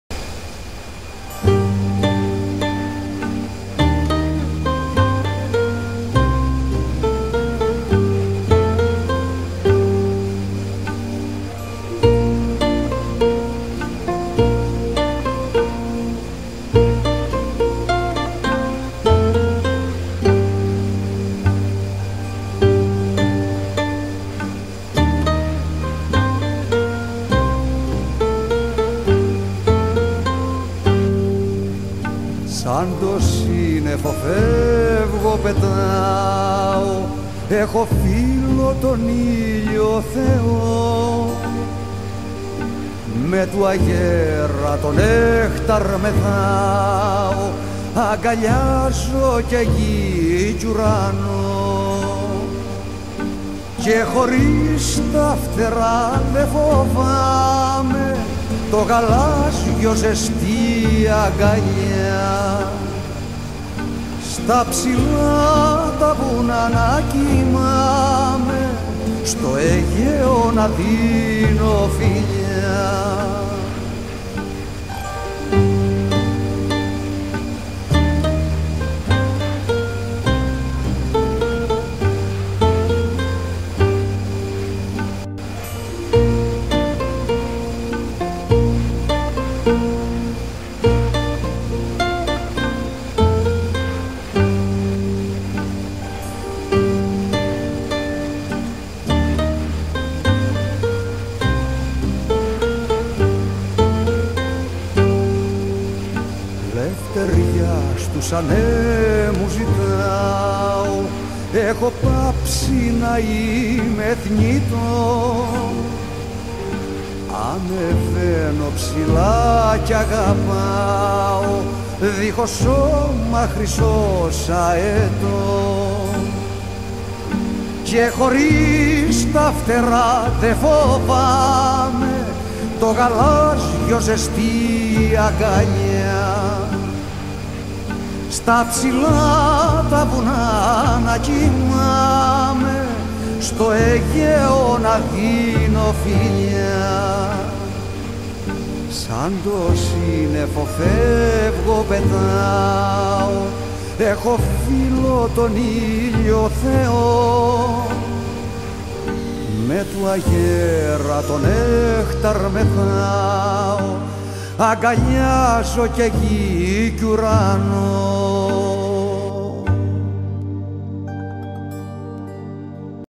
A320 landing in Simferopol
Landing in Simferopol during a tour with AegeanVA.